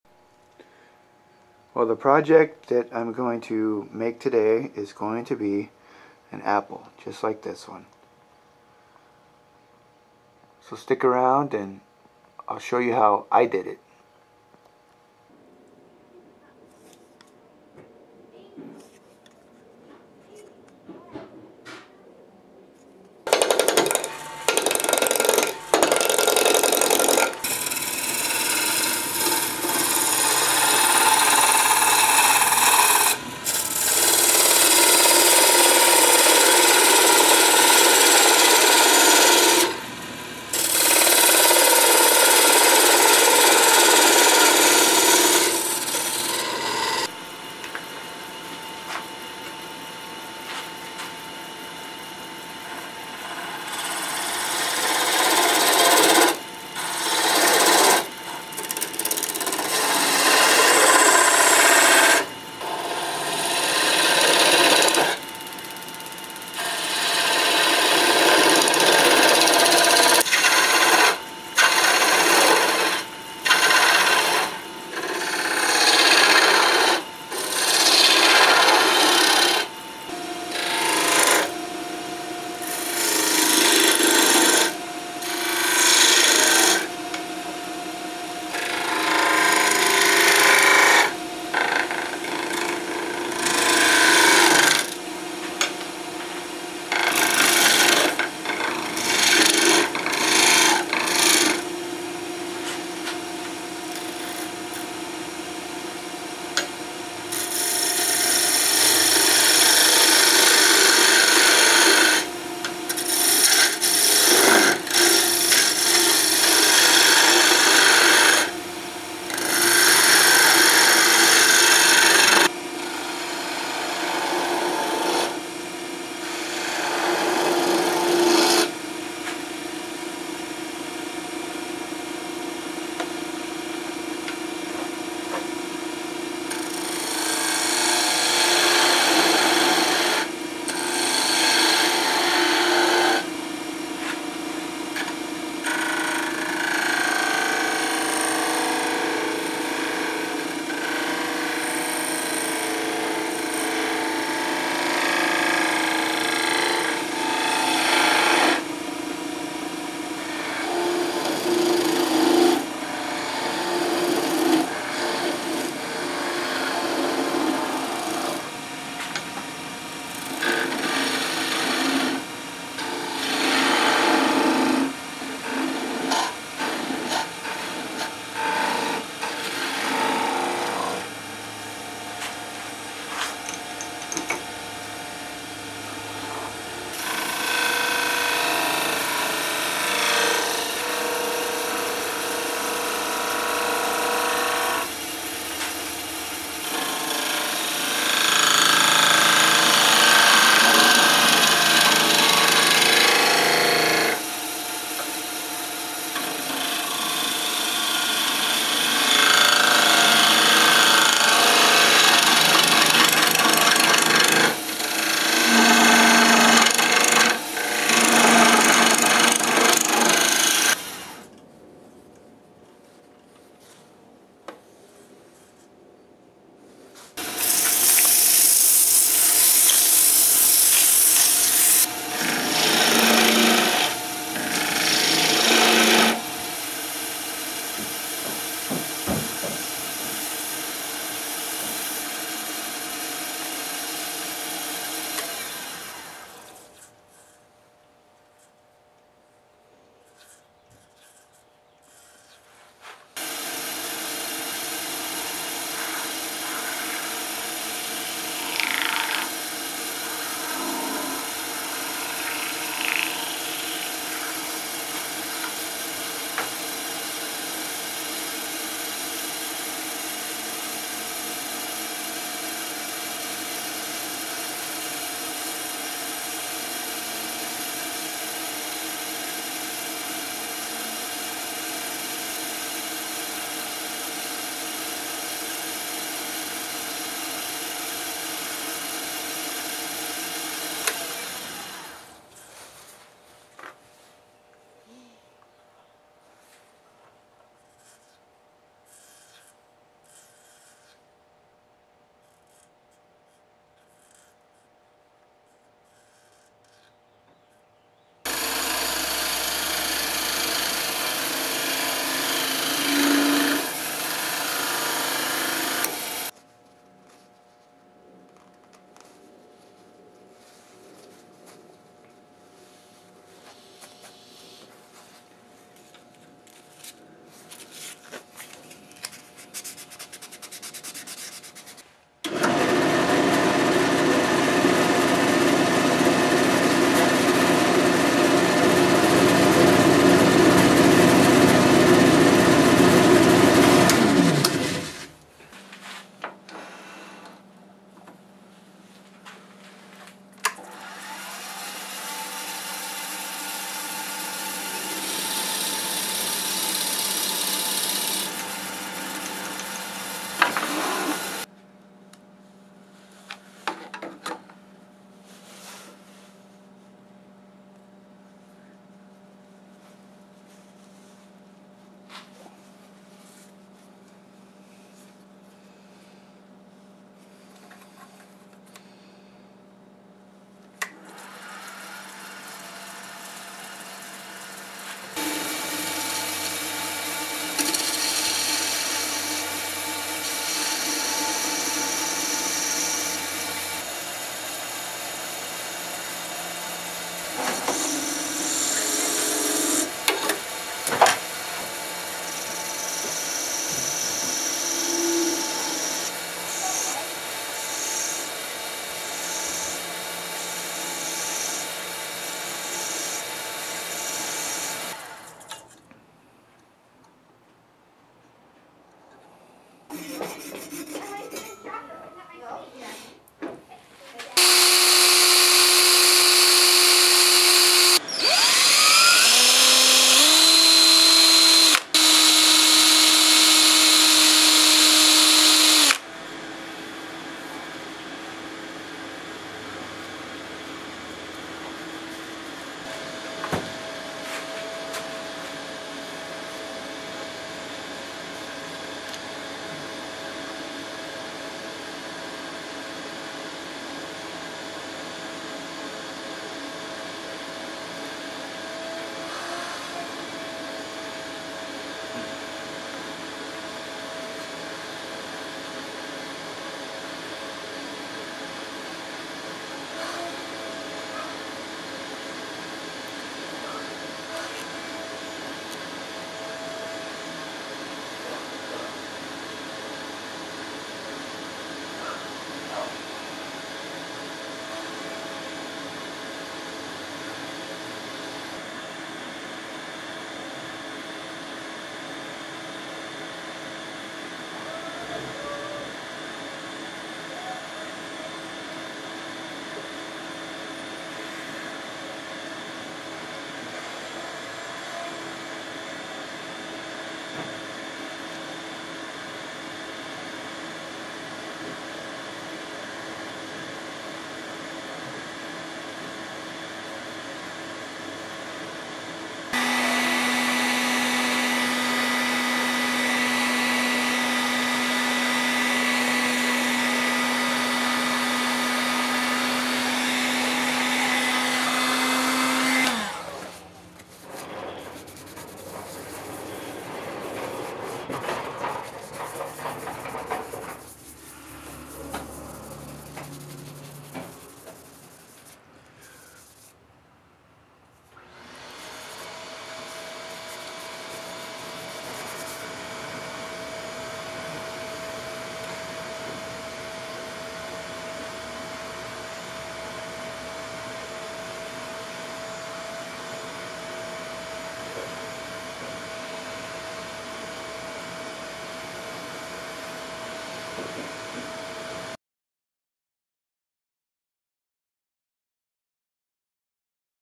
Wooden Apple
An apple turned from cherry wood with an ebony stem.  It's a pretty simple project for the lathe and it was FUN!